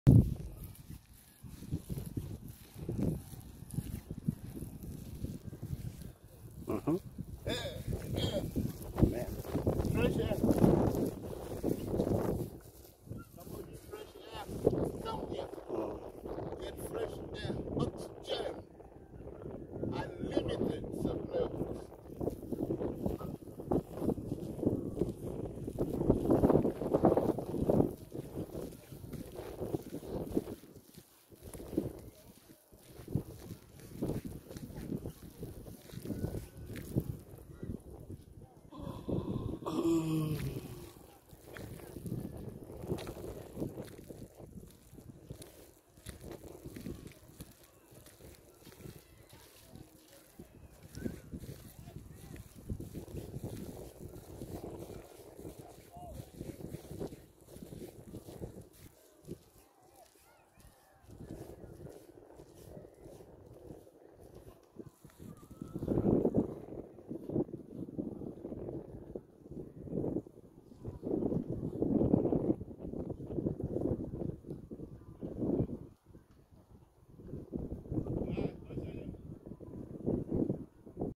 MANGROVE ECOSYSTEM CONSERVATION
we planted over 3000 mangroves at Jomvu Kuu,  Mombasa in an effort to conserve the mangrove ecosystem